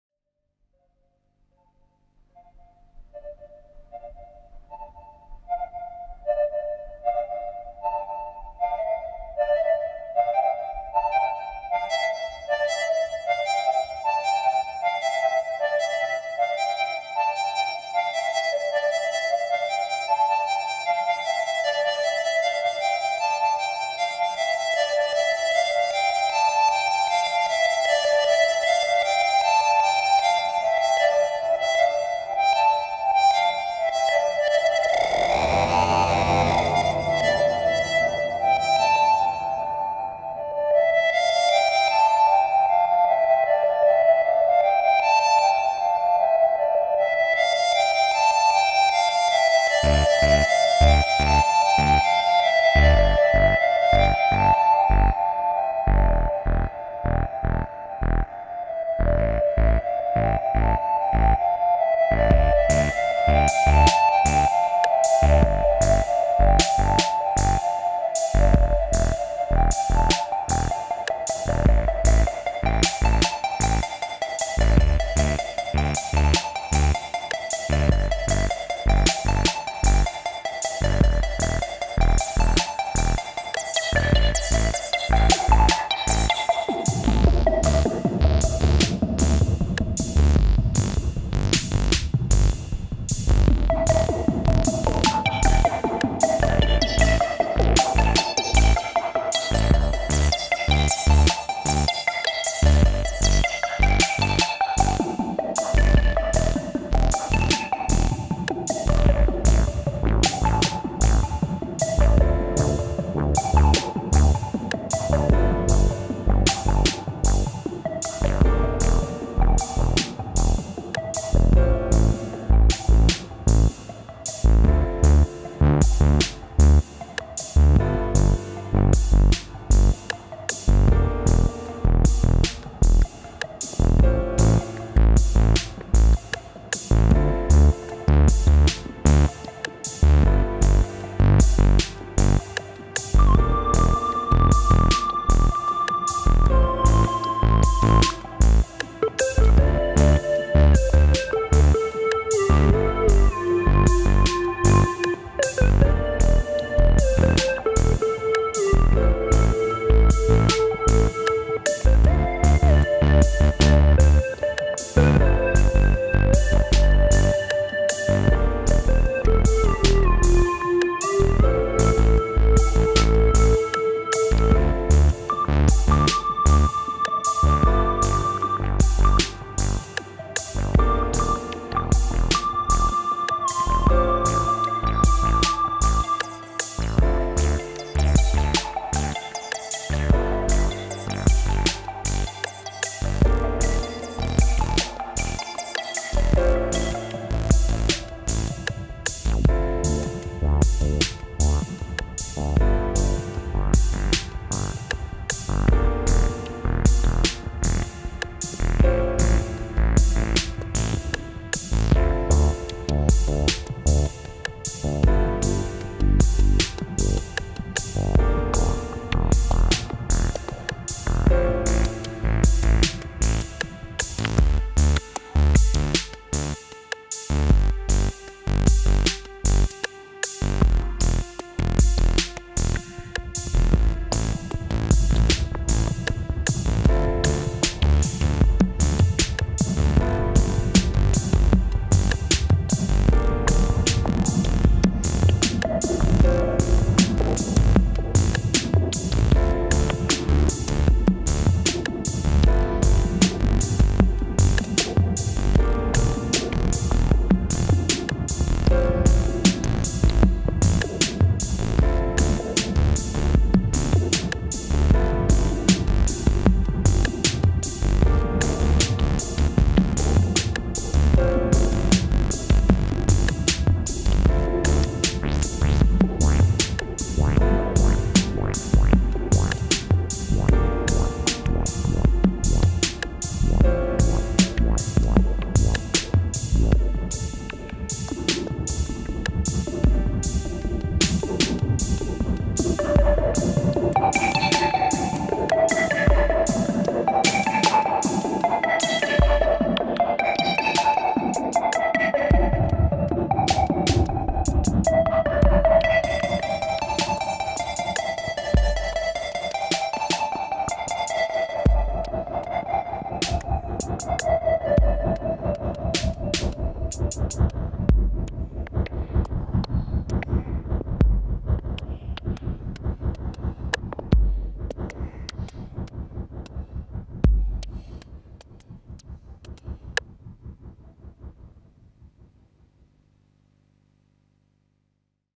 SUNDAY AFTERNOON 1 - ein kleines Schrittchen weiter....
(fast) ohne Rechner :-) - Mangels Hardware Sequenzer kommt das Piano aus Ableton...
Ansonsten: Behringer Model D für den Bass / Volca Keys und Deepmind für die Arpeggios / Funkbox (iPad) als Drummachine und B-Steop Pro (iPad) als Sequenzer für den Bass